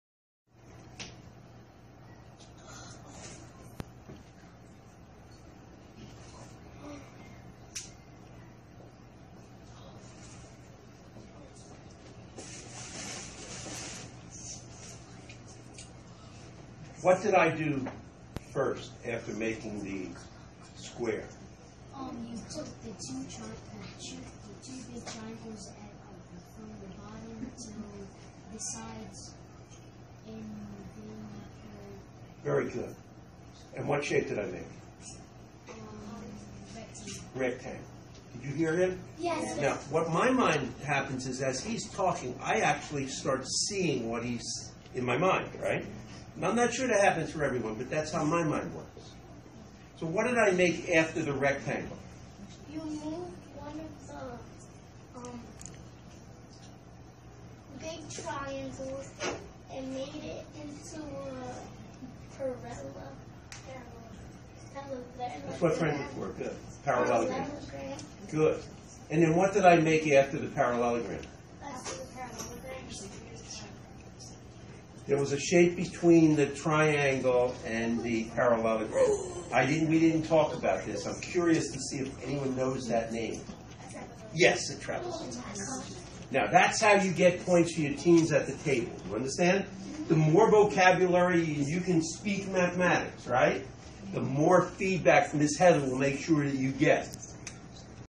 Young Consumer Warner Elementary visit 11/7/2014- Coach teaches variation of basic shapes
The Kenny Family Foundation has partnered with Warner Elementary School with a Young Consumer education grant for the 2014/2015 school year. We are committed to engage the students and help improve the scores of the 4th grade students.  In this video Coach shows the shape manipulation and the importance of speaking mathematics.